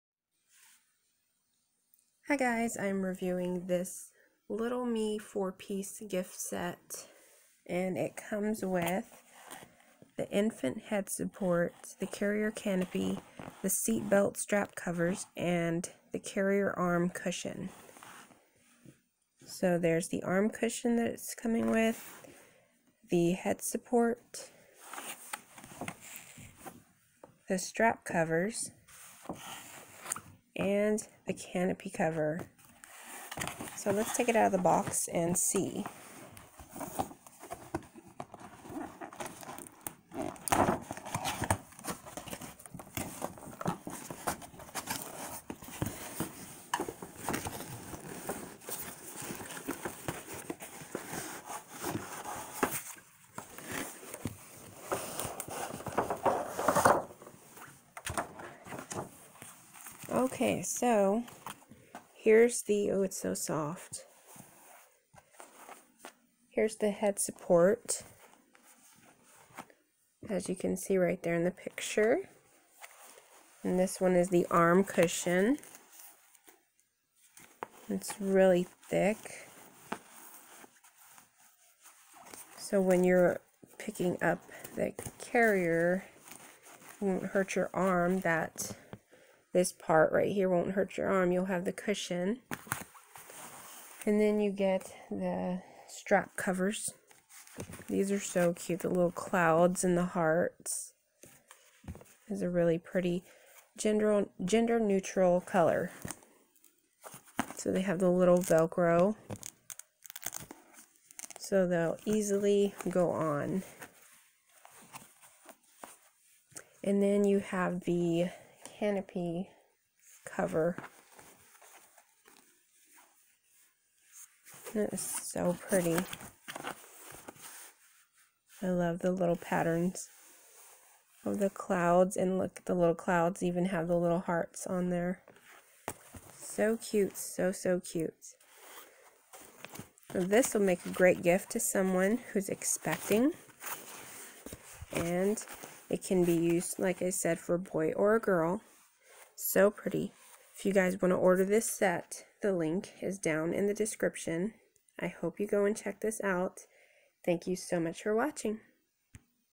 Little Me 4 Piece Baby Carrier Set Review
This car seat set comes with a head support, arm cushion, strap covers and a canopy cover. It's gender neutral colors so it's perfect for a boy or girl. The material is really soft and comfortable especially the head support.